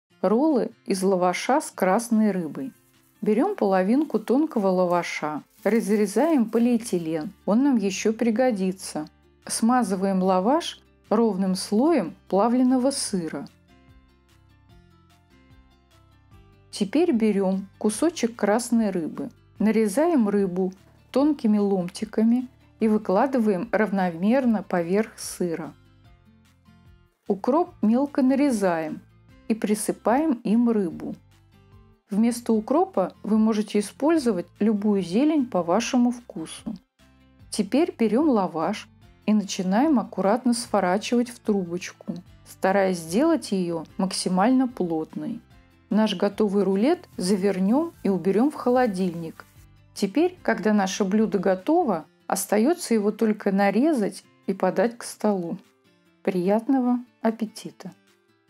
Рулет из лаваша с красной рыбой/ Лаваш с семгой и плавленым сыром
Ингредиенты:

- сёмга холодного копчения или слабой соли ~ 250 г.
- плавленый сыр ~ 200 г.
- тонкий лаваш /ролл
- зелень

ROLL OF PITA BREAD WITH RED FISH/ ROLL OF PITA BREAD/ PITA BREAD WITH SALMON

Recipe:

- cold smoked salmon ~ 250 g
- melted cheese ~ 200 g
- thin pita bread
- dill